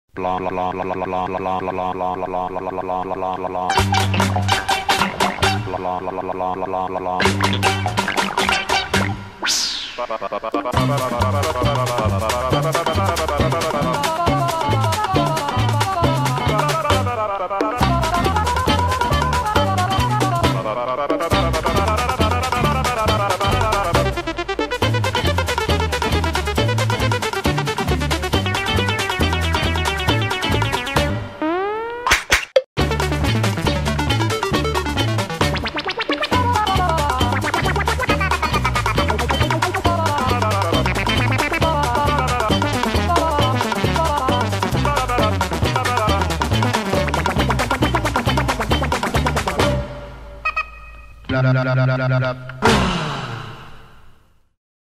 CRUMAR
Crumar plays a song and sings the song.